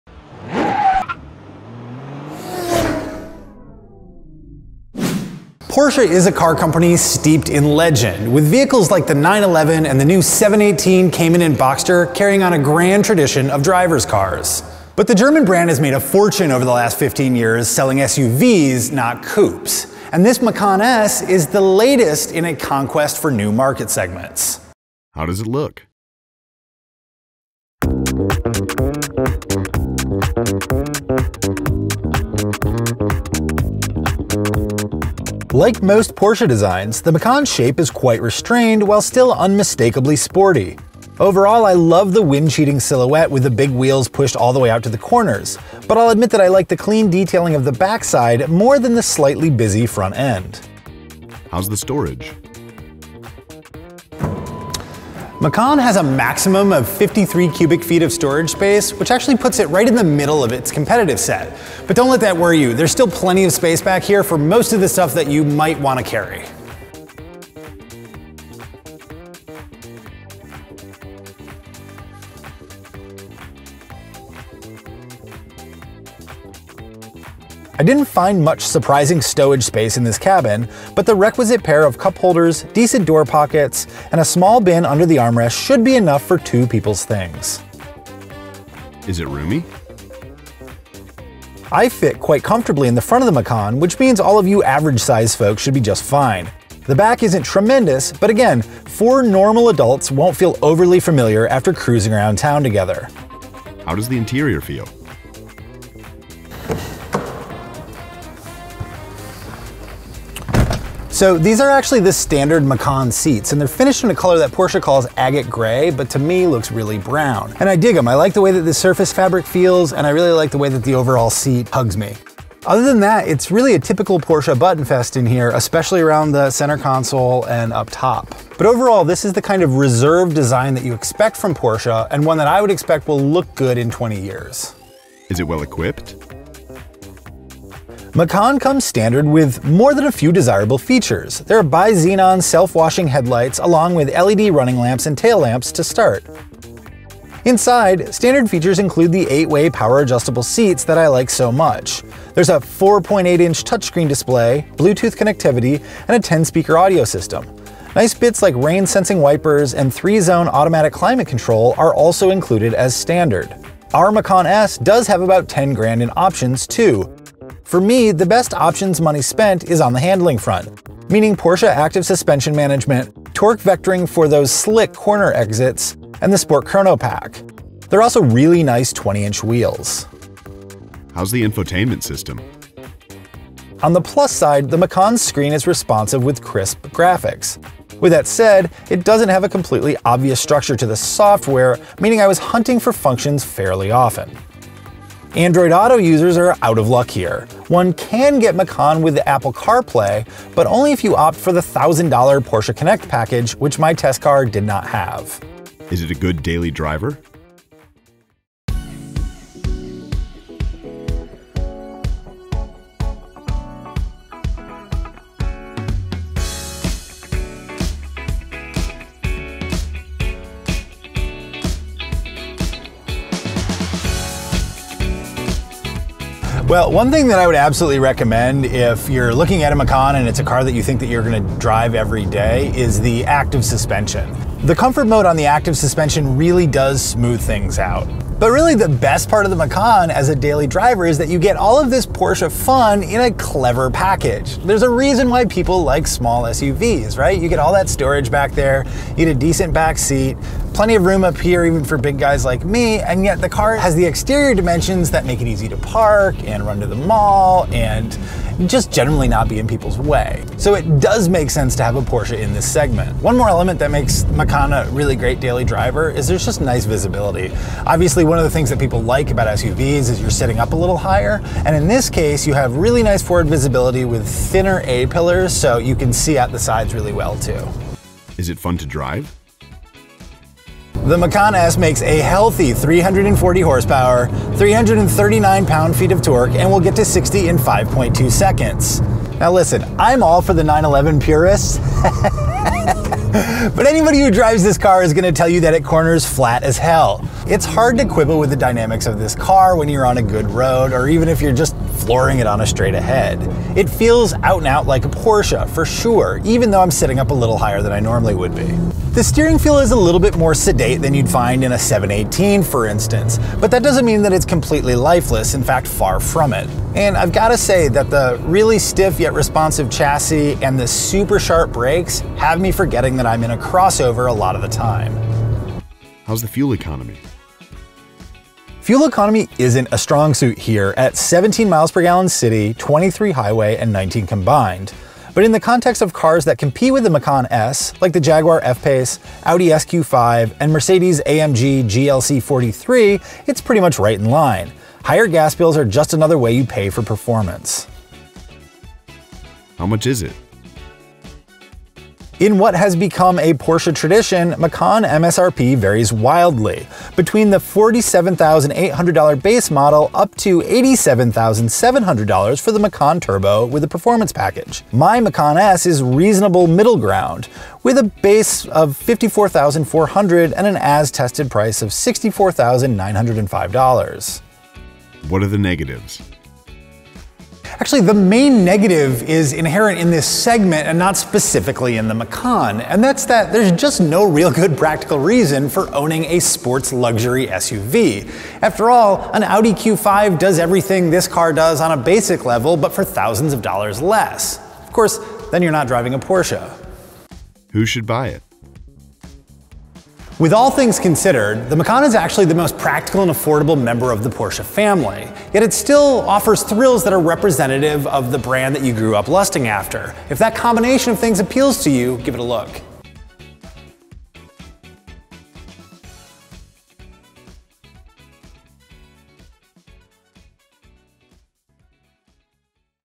Why Buy? | 2017 Porsche Macan S Review
Motor1 Executive Editor Seyth Miersma tests out Porsche’s pint-sized SUV offering to see if a compact crossover can do justice to the storied brand.

There was a time when every vehicle to wear a Porsche badge was a sports car. Today, while some of our very favorite sons of Stuttgart still fit that description, the German maker sells many more SUVs than it does coupes. That’s a fact that the purists still bemoan, but probably because they haven’t actually driven something like this Macan S, which is a driver’s car no matter how high it is off the ground.

With a turbocharged 3.0-liter V6 developing 340 horsepower and 339 pound-feet of torque, the Macan S has power enough to make it entertaining in a straight line. But, as we find out in this week’s edition of Why Buy? the real fun starts where the road gets twisty – a typical Porsche trait but not one you might expect from a vehicle in this class.

Watch the video above to see how Porsche’s utile vehicle stacks up against the likes of the Jaguar F-Type and Audi SQ5, along with the typical reporting on fit, finish, space, and the like.

SPECIFICATIONS:
Engine: Biturbocharged 3.0-Liter V6
Output: 340 Horsepower / 339 Pound-Feet
Transmission: 7-Speed DCT
EPA Fuel Economy: 17 City / 23 Highway / 19 Combined

Like our fancy new luggage? to buy a set for yourself.

CREDITS
Host: Seyth Miersma